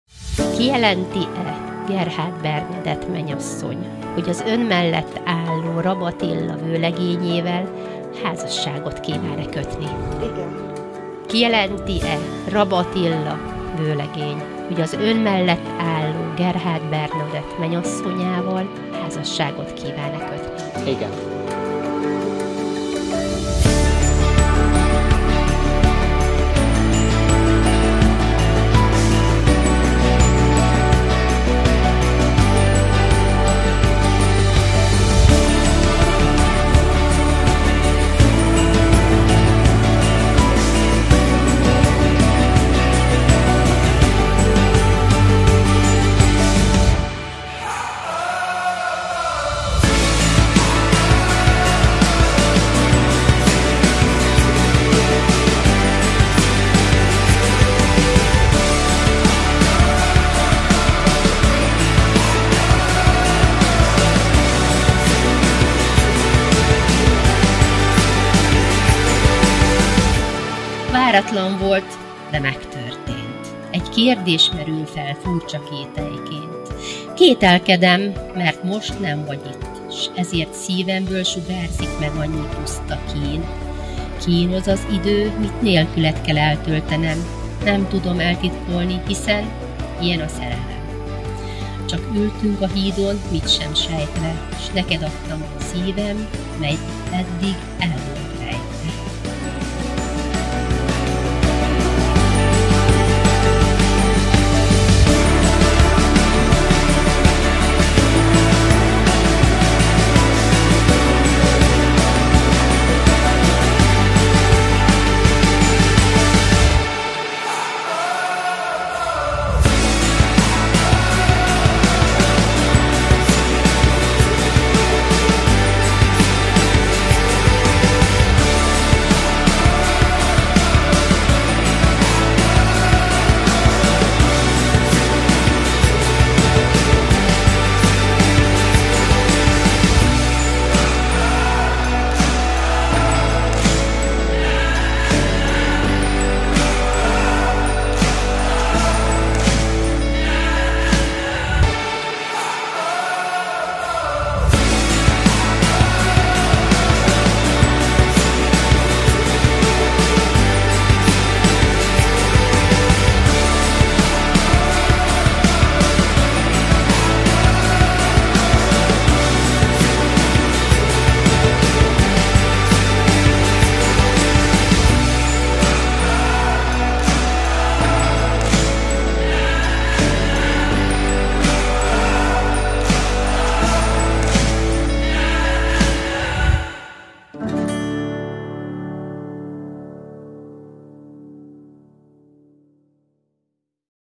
B&A Highlights (2021.09.10)
Bernadett és Attila nagy napja.

Szellő Lovastanya (Domony)
Fotó: Kelemen Gergő, Steirer Matt
CM/Vőfély: Szőllősi Roland
DJ/Zenekar: Kratancsik Zsolt (Sense Studio)